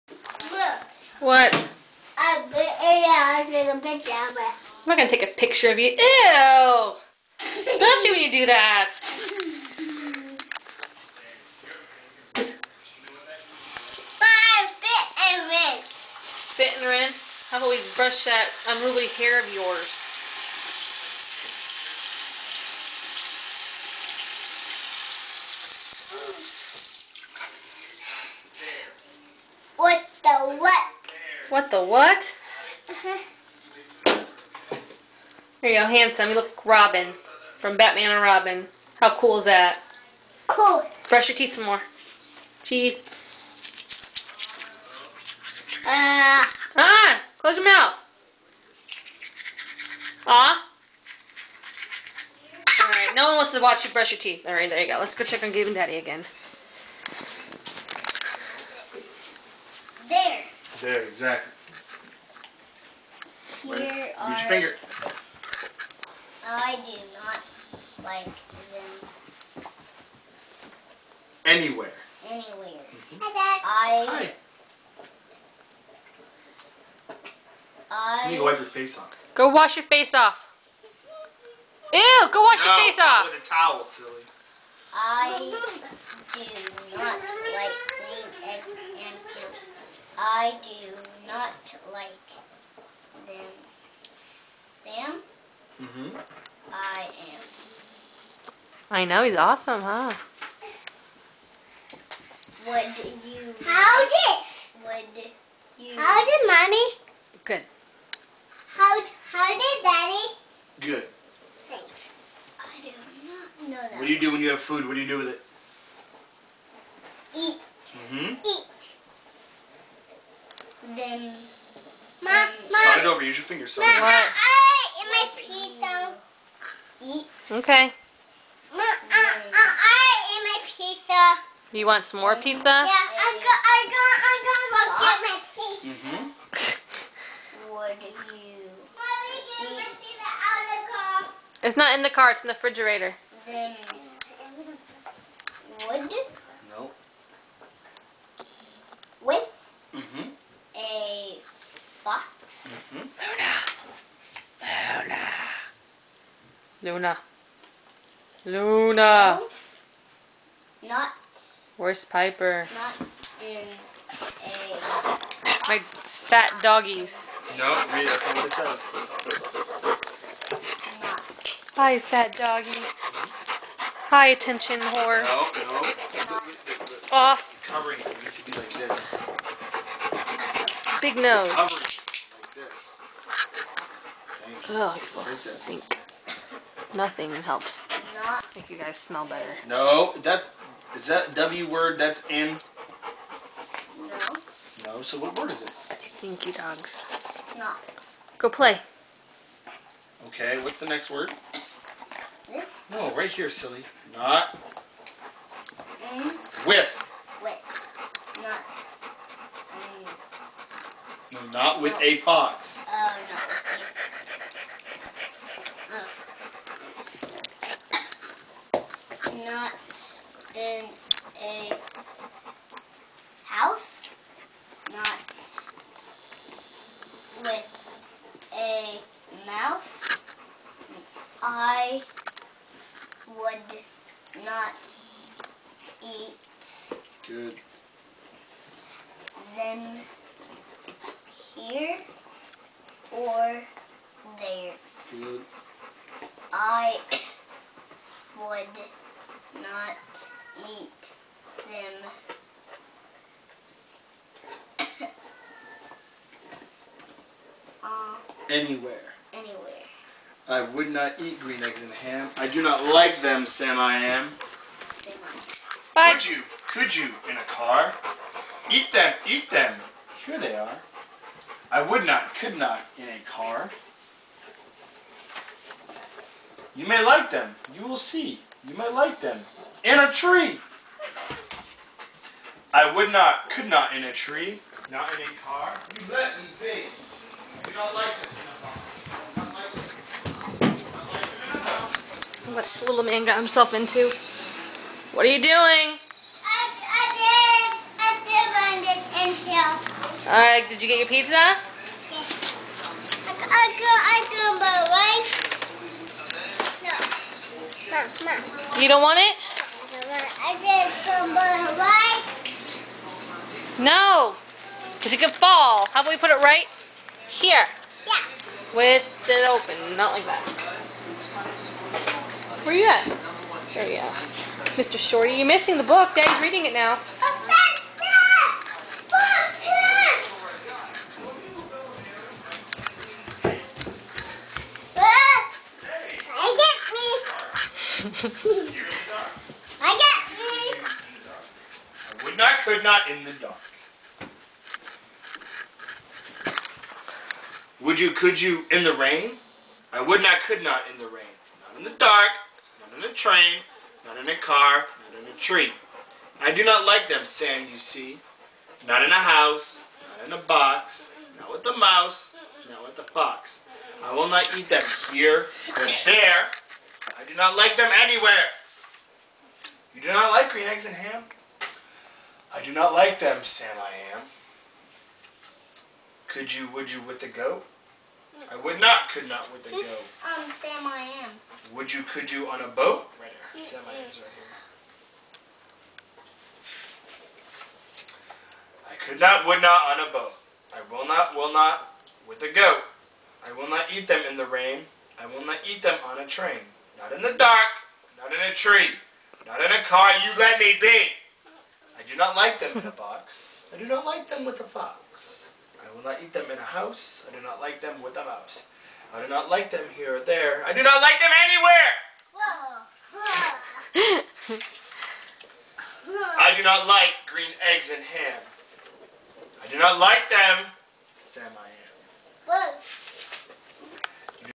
Dan reads to the boys
A little bit of reading and mischief :)